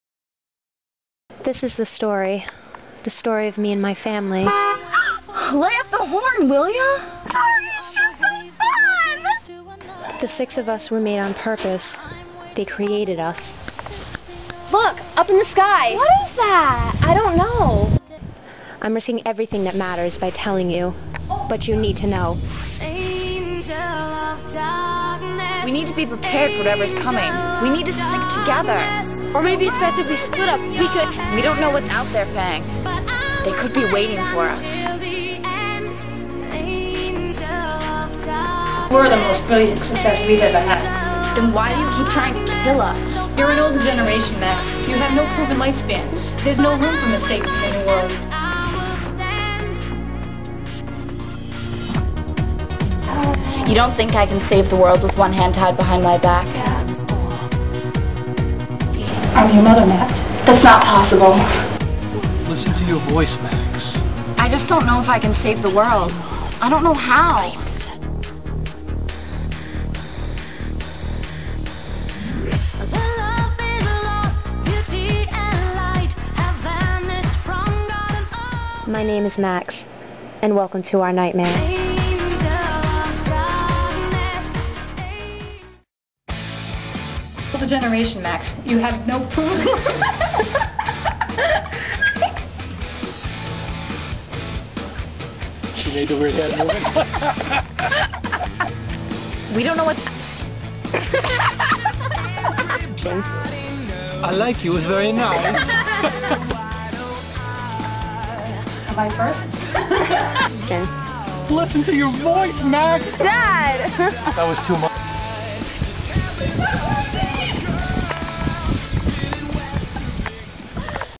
Maximum Ride 3 Trailer (not really)
(taken from my old account)

this is the story, the story of me and my family...

Director/ Max's voice/ Girl 1: Jenn Sadowski
Max: Sarah Doucette
Nudge/ Girl 2: Katie Ramos
Fang: Bryan Sadowski
Max's Mom: Pat Sadowski
Jeb: Richard Sadowski

Production Company: Jenarah Pictures